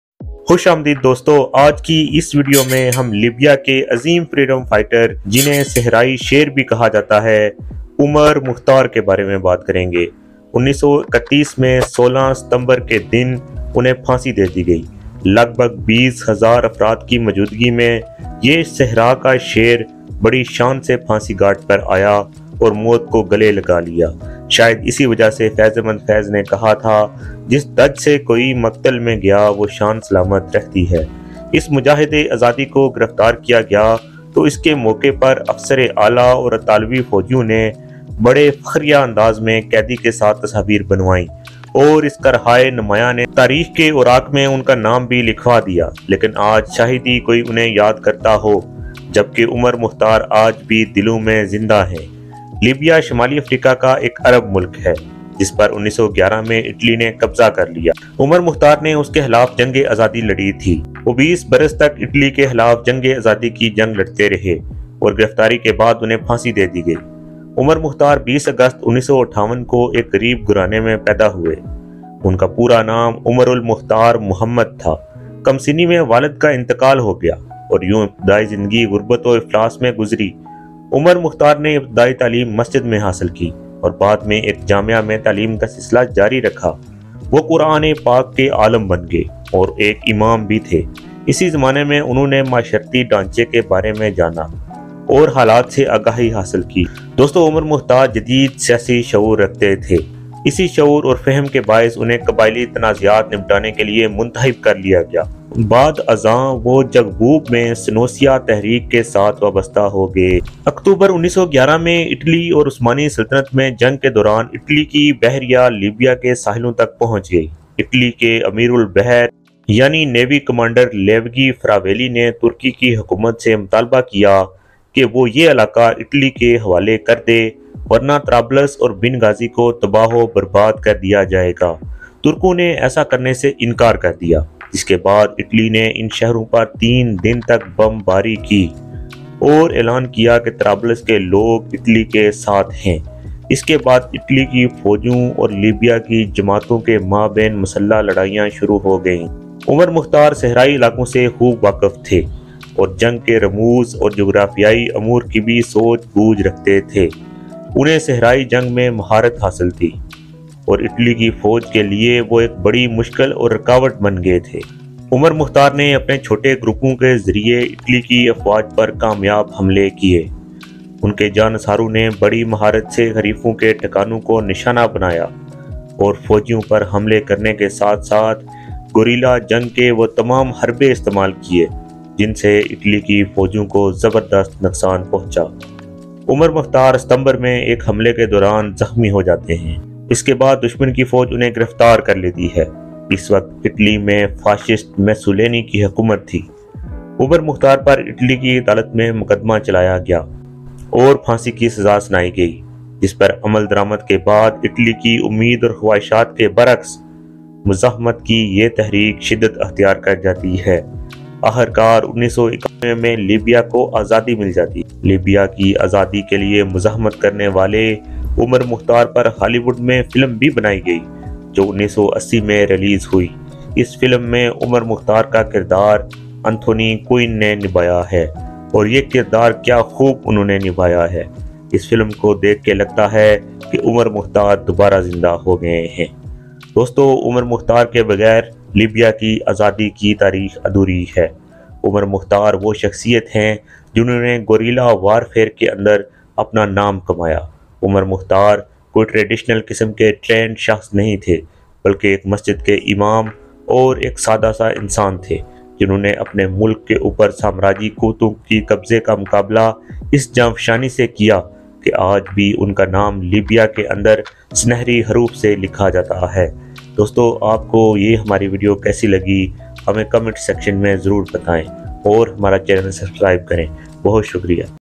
Who is the Lion Of the desert? Libyan Freedom Fighter Omar mukhtar complete biography in Urdu/Hindi
لیبیا کی آزادی کا وہ ہیرو جس نے غاصب اطالوی افواج کو دھول چٹا دی، اس کی بہادری اور پھرتی پر اسے صحرا کے شیر کا خطاب دیا گیا، دنیا بھر کے انقلابیوں اور حریت پسندوں کے  ہیرو۔۔۔۔ لازمی دیکھیں
عمر مختار اپنی پھرتی اور چالاکی کی وجہ سے مشہور تھے، وہ نہ صرف صحرا میں جنگ لڑنے کے ماہر تھے بلکہ وہ ایک عظیم سپہ سالار بھی تھی، آزادی کی جنگ کے وہ ہیرو تھے۔